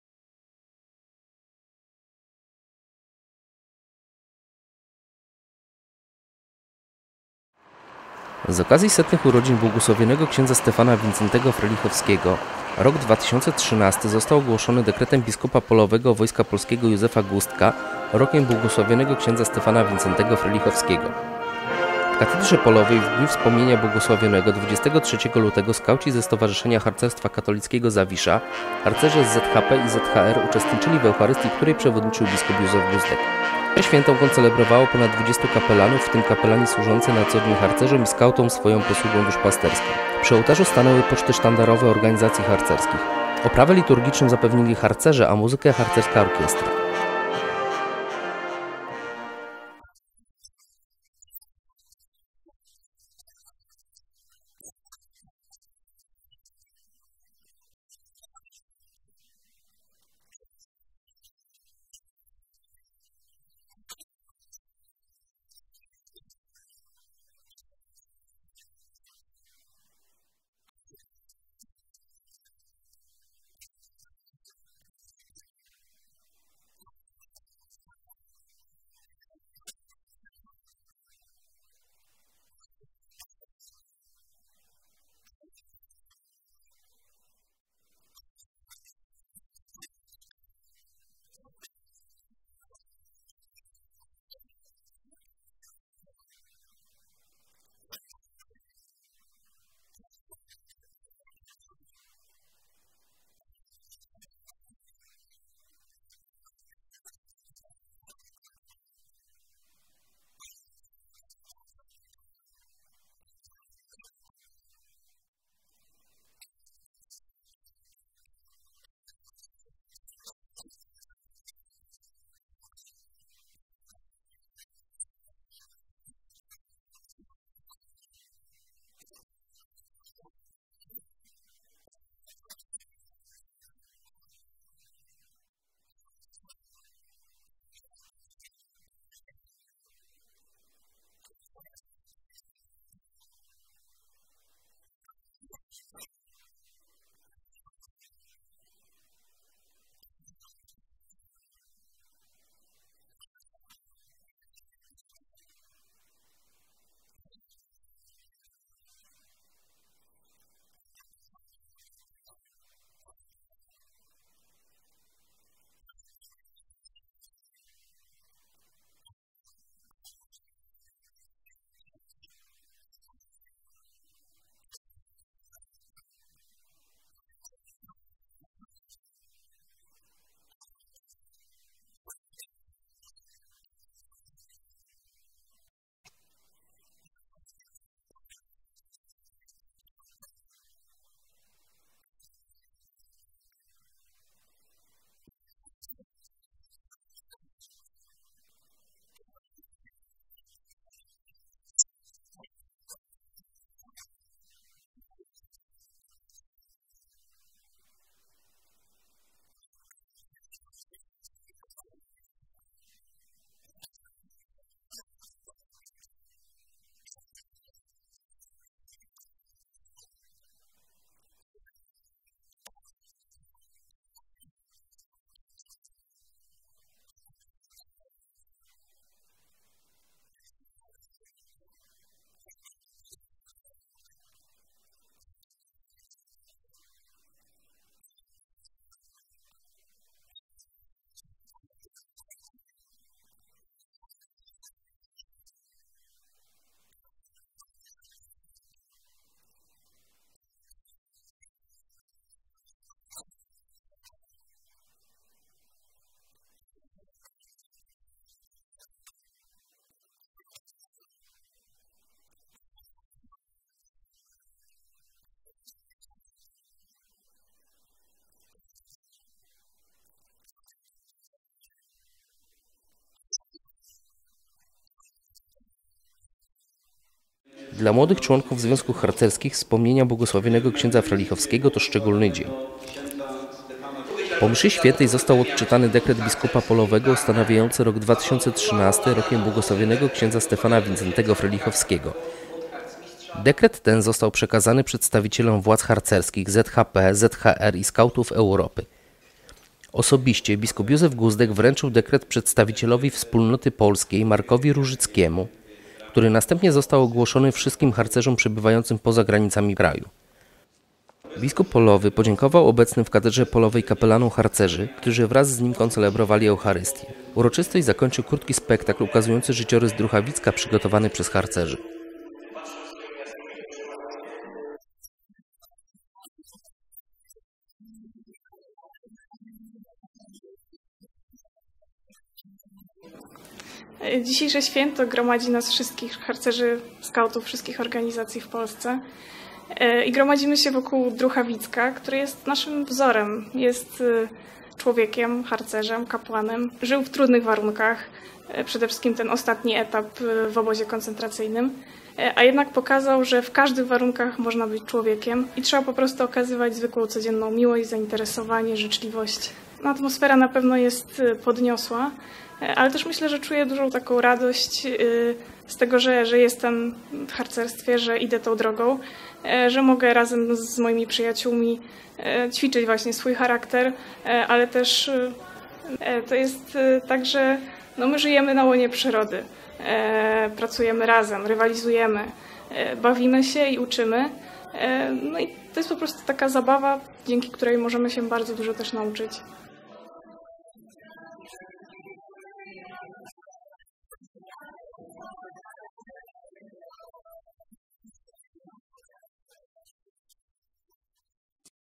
Harcerze modlili się za wstawiennictwem bł. ks. Wincentego Frelichowskiego
Z okazji setnych urodzin bł. ks. Stefana Wincentego Frelichowskiego rok 2013 został ogłoszony dekretem biskupa polowego WP Józefa Guzdka rokiem bł. ks. Stefana Wincentego Frelichowskiego. W Katedrze Polowej w dniu wspomnienia błogosławionego, 23 lutego, skauci ze Stowarzyszenia Harcerstwa Katolickiego „Zawisza" (Skauci Europy), harcerze z ZHP i ZHR uczestniczyli w Eucharystii, której przewodniczył bp Józef Guzdek.